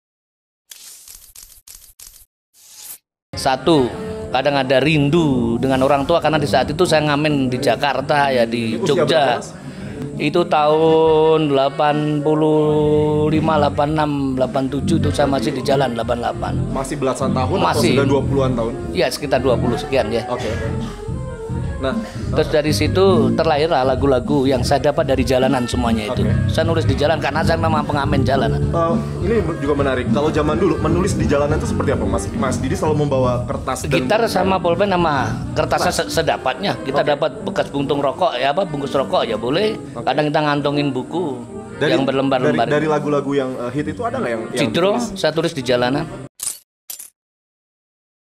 Perjalanan Karir Didi Kempot, Sempat Menulis Lagu di Bungkus Rokok
Didik Prasetyo, atau yang akrab disapa Didi Kempot merupakan musisi Indonesia yang bergelut dalam musik campursari ataupun pop dangdut. Sebelum menuju puncak kesuksesannya, Didi Kempot merupakan seorang pengamen jalanan dan kerap pindah-pindah kota. Namun tak ada yang menyangka, curahan hati sang Lord Didi yang kerap ia tulis dimana saja hingga menjadi lirik lagu, dapat mengantarkan dia menjadi sosok musisi yang terkenal hingga saat ini.